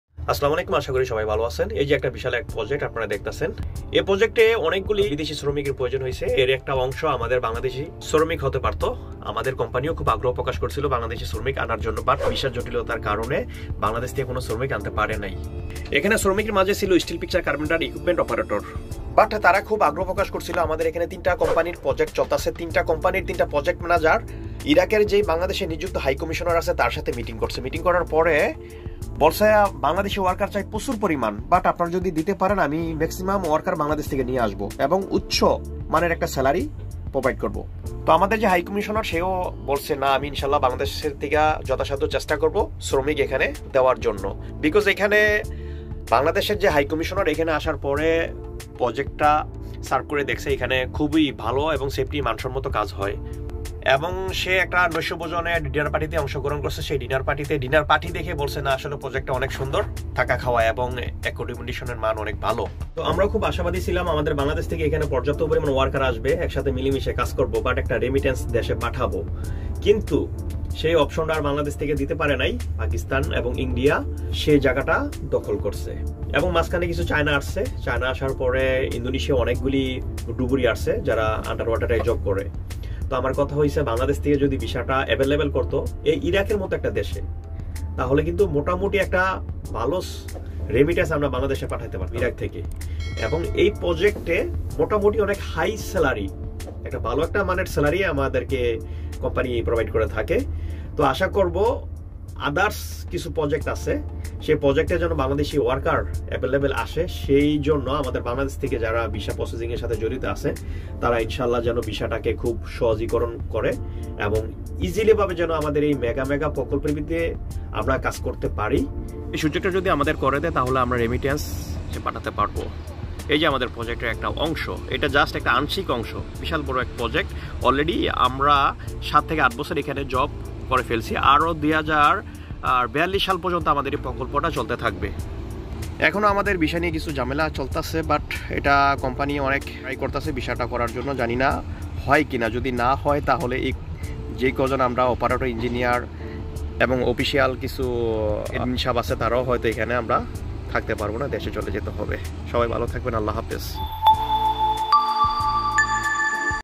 হেভি ইকুবমেন্ট অপারেটর ও মেকানিকের ভালো চাহিদা || Equipment Operator & Mechanic
➤ইরাকে আমাদের বাংলাদেশীদের ভিসা বন্ধ কিন্তূ ইরাক সরকার অবৈধ বাংলাদেশিদের বৈধ করবে,
তার মানে ভিসা প্রদান করা হবে,

☞তো আমার কথা হচ্ছে নতুন করে যদি বাংলাদেশ থেকে দক্ষ শ্রমিক ইরাকে আসা যায় তাহলে বাংলাদেশে ভালো একটা রেমিটেন্স পাঠানো সম্ভব হবে।

☞এদেশে কোরিয়ান কোম্পানিতে বাংলাদেশি ওয়ার্কারের ভালো চাহিদা আছে!

চাইলে ফেইসবুক পেজ ফলো করে রাখতে পারেন।